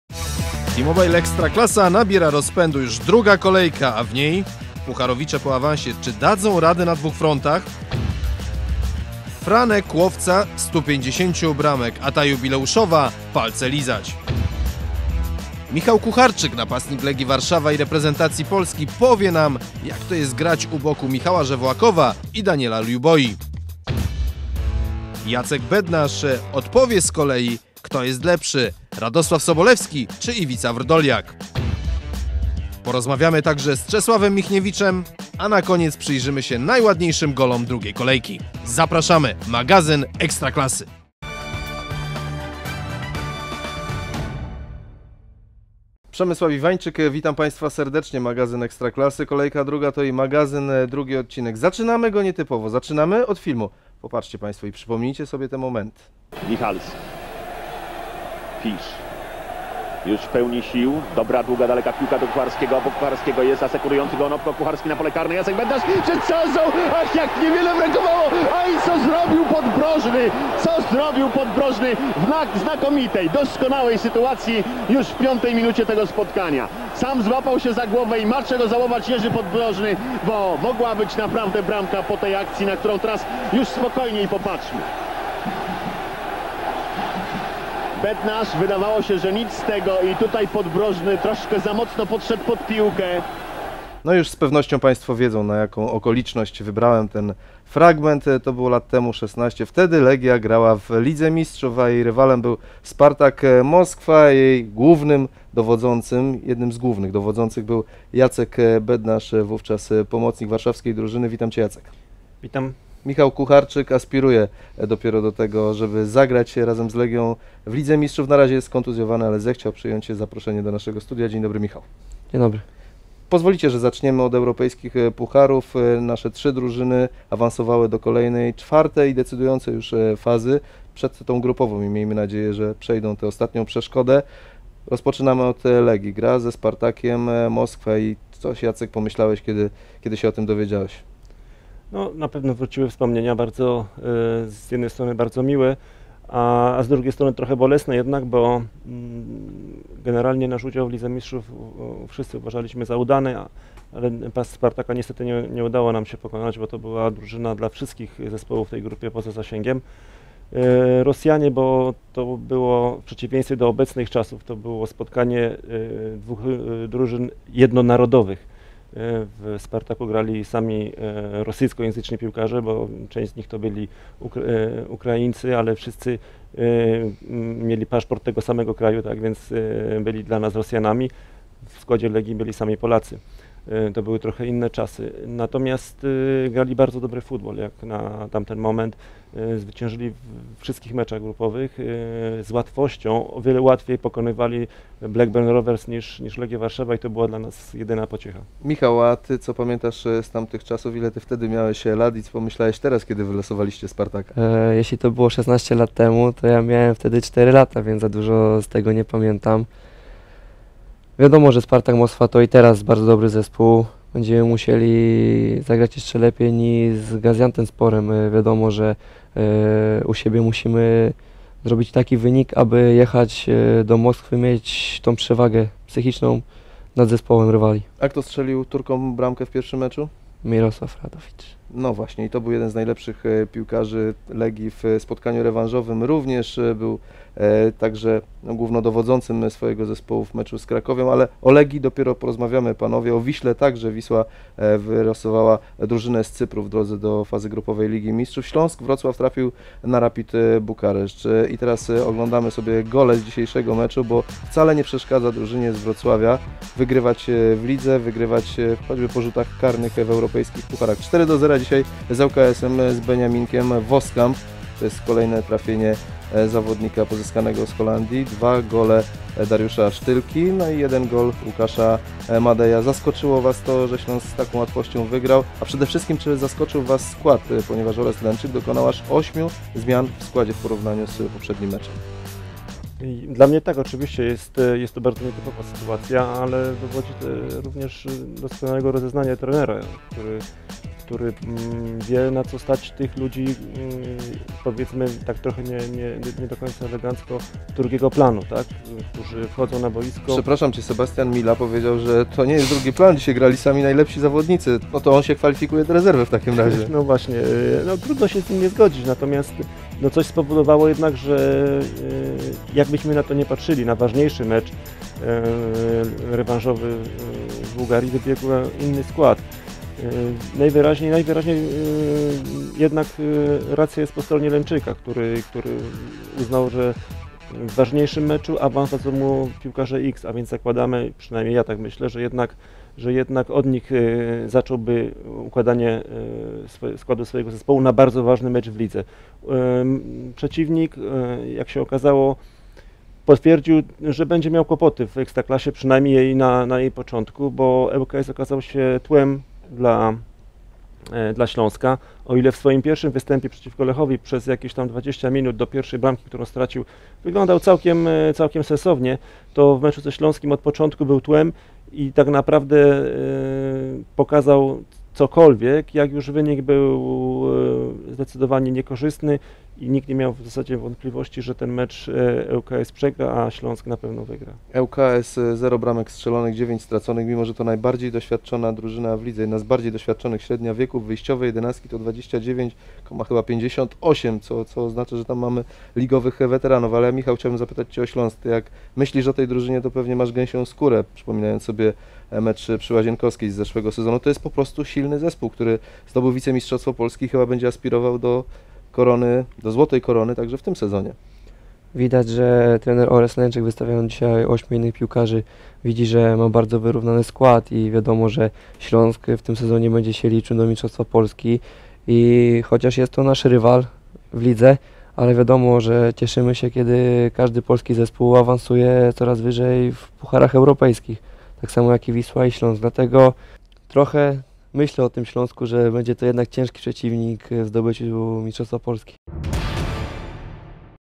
Napastnik Legii: Śląsk idzie na mistrza
Ekstraklasa.tv: Jacek Bednarz, były pomocnik Legii, wspomina sobie swoje pucharowe występy przeciwko Spartakowi Moskwa sprzed 16 lat i ocenia szansę obecnej drużyny z Łazienkowskiej w starciu z Rosjanami w eliminacjach Ligi Europejskiej. Michał Kucharczyk, napastnik Legii i reprezentacji Polski miał w 1995 roku zaledwie cztery lata i zastanawia się, jak występy w Europie wpłyną na ligę. Śląskowi to nie przeszkadza, który wygrał z ŁKS 4:0, a trener Orest Lenczyk wymienił aż ośmiu piłkarzy z podstawowej jedenastki. - Racja jest po stronie Lenczyka - przyznaje Bednarz. - Choć to nasz rywal, Śląsk to kandydat do mistrzostwa - uważa Kucharczyk. Więcek wideo